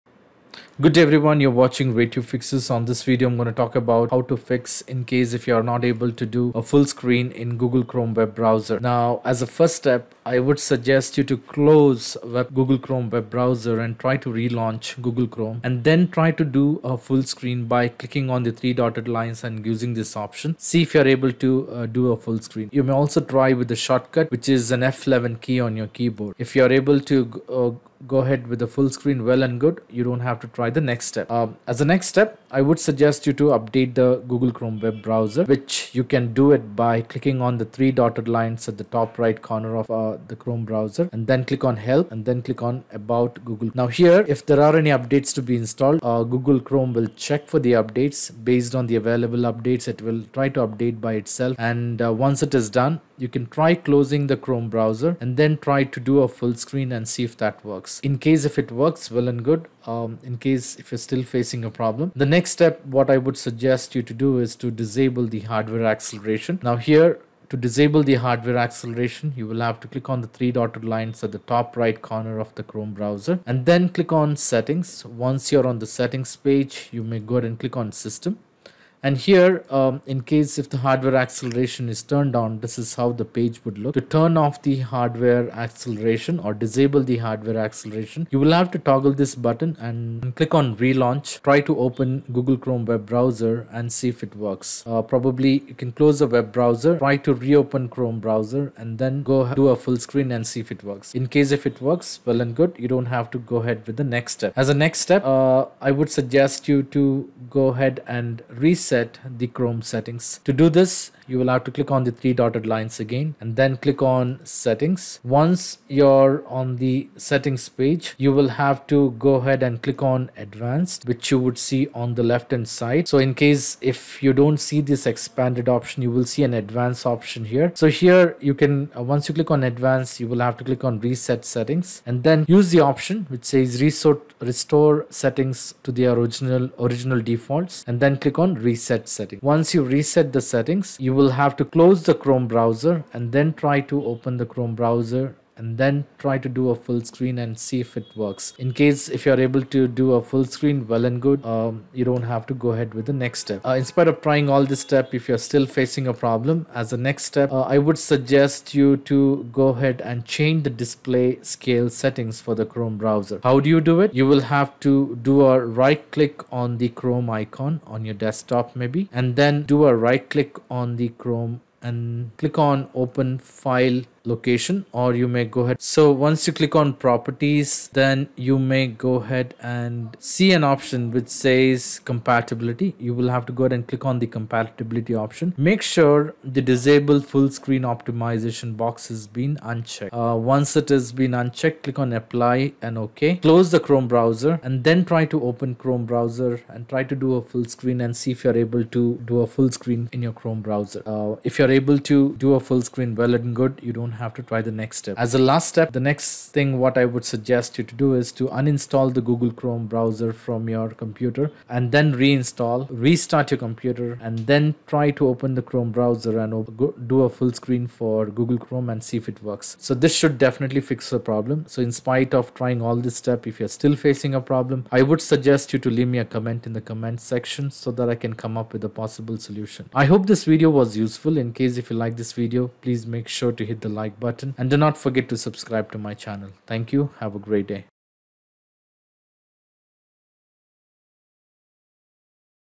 FIX!!! Full Screen Not Working In Google Chrome F11
Google Chrome is the most-used browser in the world, with more than 50% of the market share. It’s present on all devices you could imagine and excels in many features.

However, it’s not perfect, and one of the most reported issues is related to playback and video streaming. From complaints, it seems that the Google Chrome Full-Screen mode is not working for some Windows users.

This especially goes for YouTube videos, which it may open windows that are not filling the entirety of the screen.